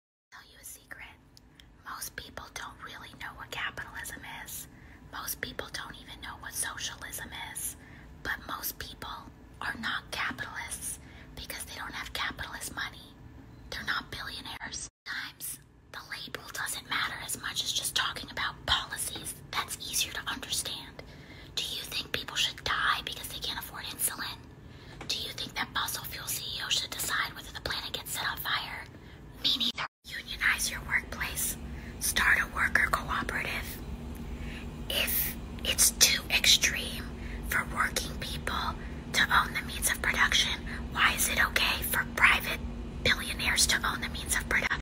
Alexandria Ocasio-Cortez - Biri sizi sosyalist olmakla suçlarsa ne yapmalısınız?
Alexandria Ocasio-Cortez Biri sizi sosyalist olmala suçlarsa ne yapmanız gerektiğini anlatıyor.